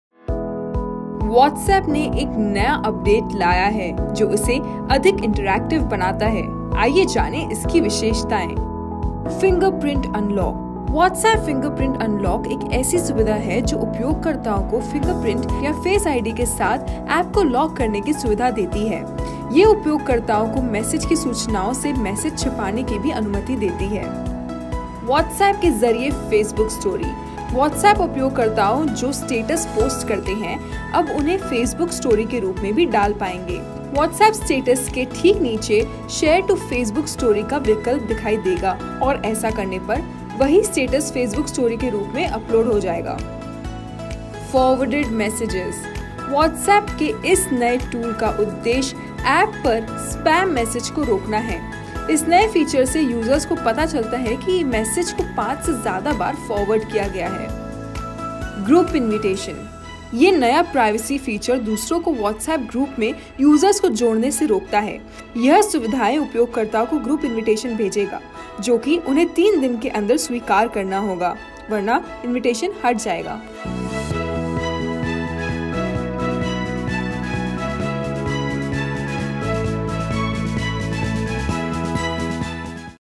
व्हाट्सऐप लेकर आया नया फीचर, तुरंत अपडेट करें ऐप...
व्हाट्सएप नया फीचर लेकर आया है। इस फीचर का इस्तेमाल आप एप को अपडेट करने के बाद कर सकते हैं। इनके बार में कई लोगों को जानकारी नहीं है। आप व्हाट्सऐप फिंगरप्रिंट अनलॉक फीचर का इस्तेमाल कर सकते हैं। इसके अलावा अब ग्रुप में आपको ऐड करने से पहले लोगों को अनुमति लेनी पड़ेगी। व्हाट्सऐप पर डाले गए स्टेटस को फेसबुक पर भी अब शेयर किया जा सकता है। व्हाट्सऐप ने ऐसे कई और अपडेट्स भी ऐड किए हैं, जो इसे और ज्यादा यूजर फ्रेंडली बना रहे रहे हैं।